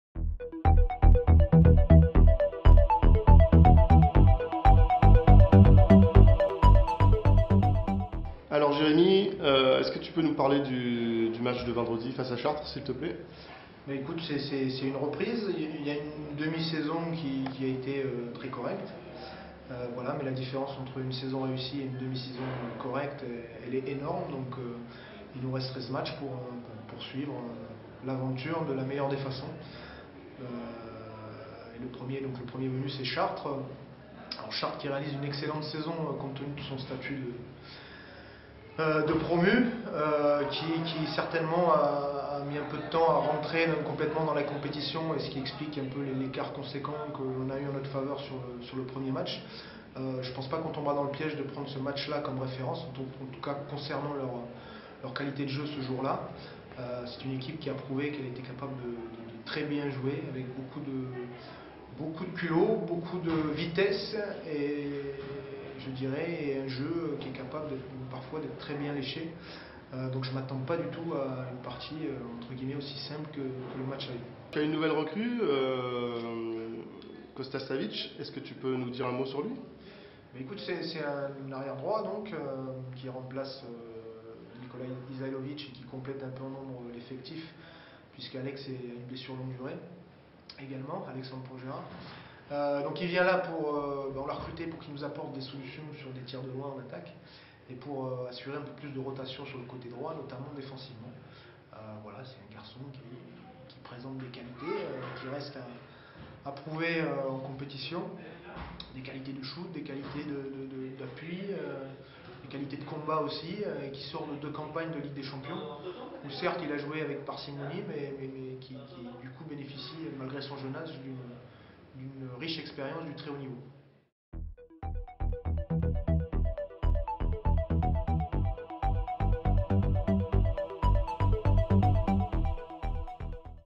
Interview de Jérémy Roussel - Avant match PAUC / Chartres
Jérémy Roussel parle du match de Pro D2 entre le PAUC et Chartres